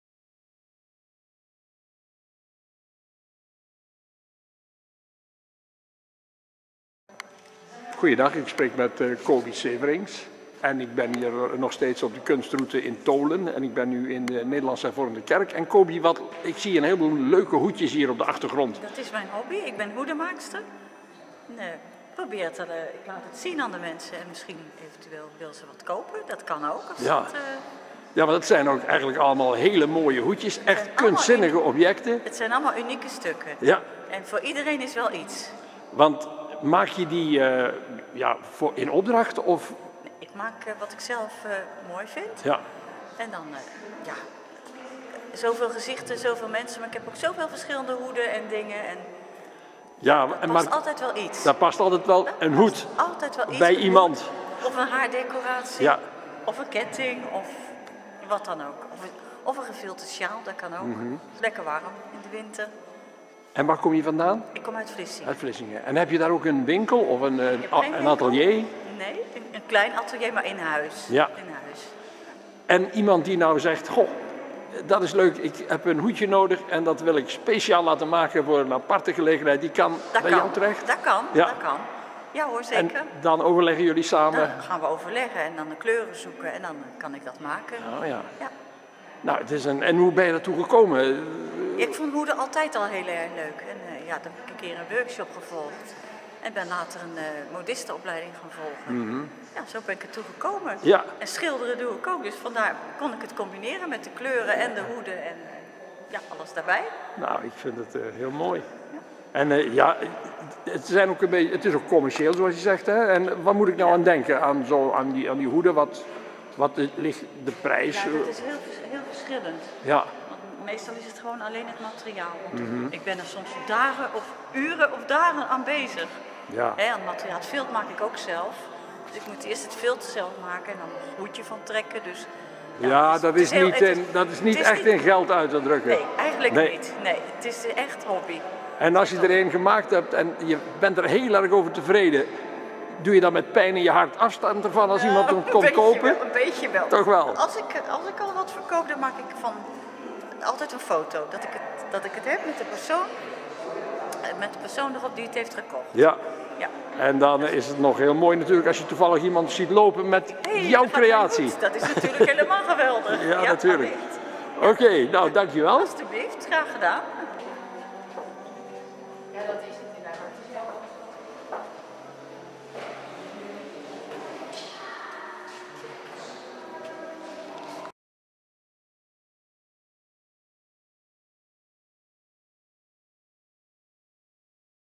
Coby Severiens Hoedenmaakster showt in Tholen
Tijdens de Kunstroute in Tholen troffen we Coby Severiens, hoedenmaakster die met passie over haar werk vertelt.